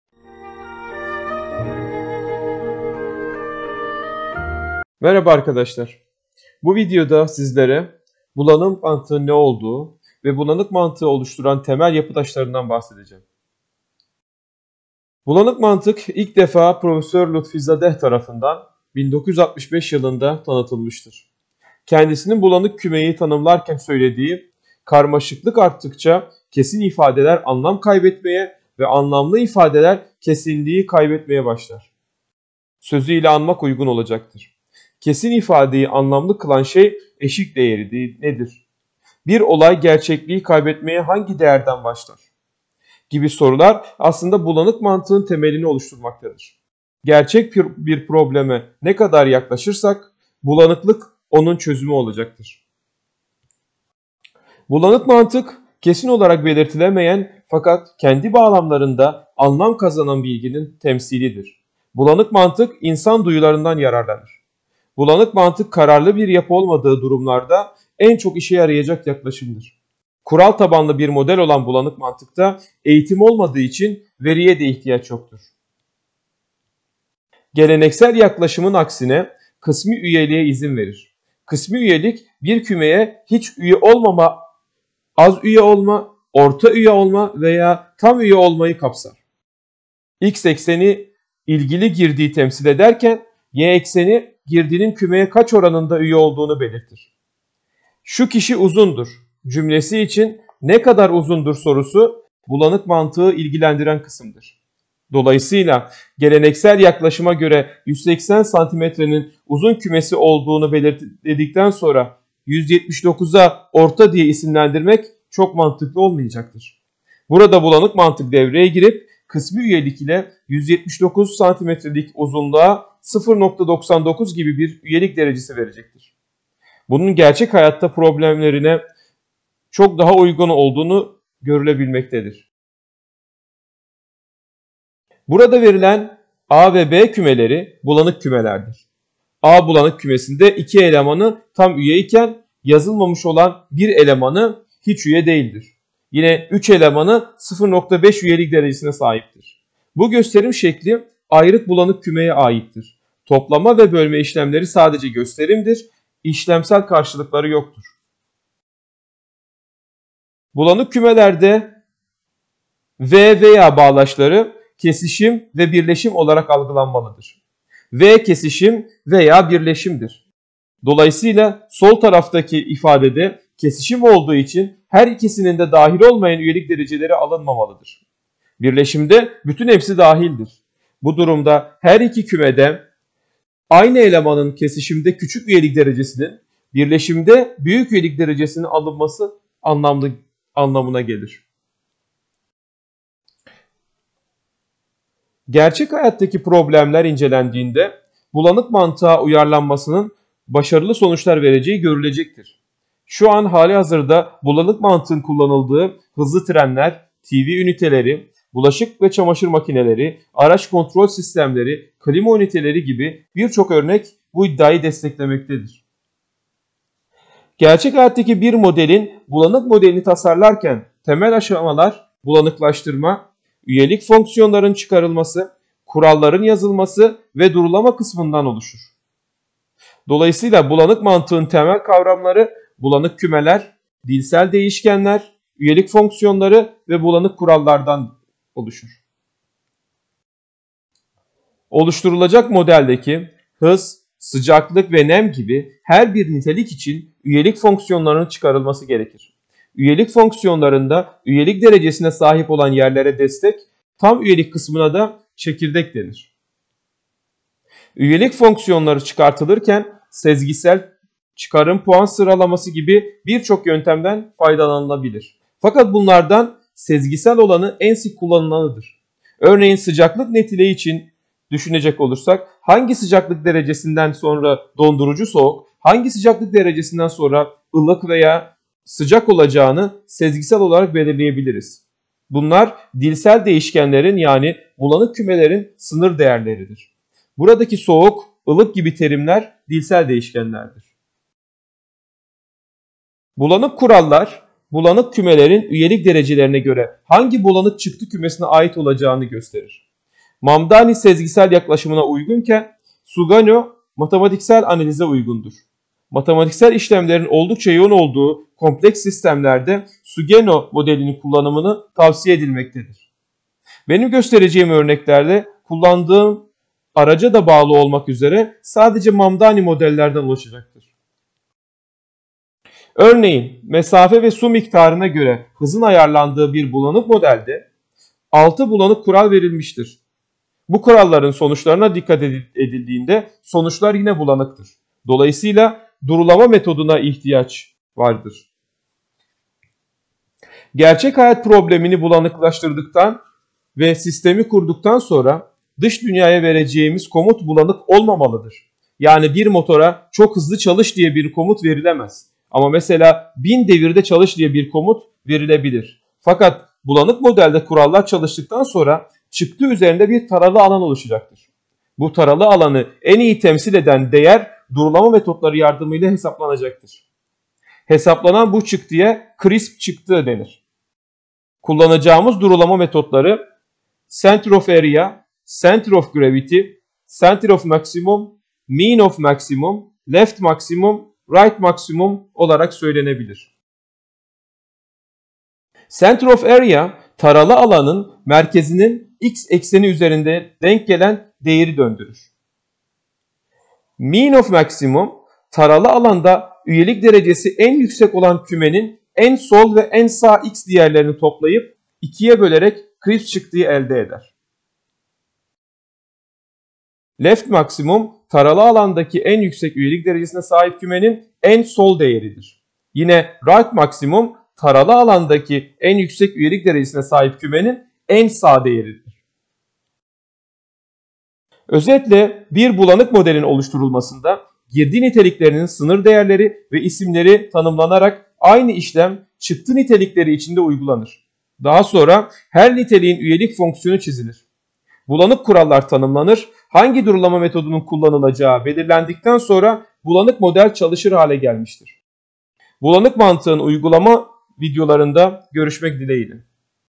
Bulanık Mantık Tanıtım ve Temel Kavramlar
Bulanık mantık nedir? Çıkış fikri nasıl oluşmuştur. Bulanık mantığı oluşturan temel yapı taşları, geleneksel yaklaşım ve bulanık yaklaşım,
Bulanık kümeler,
Dilsel değişkenler,
Üyelik fonksiyonları
Bulanık kurallar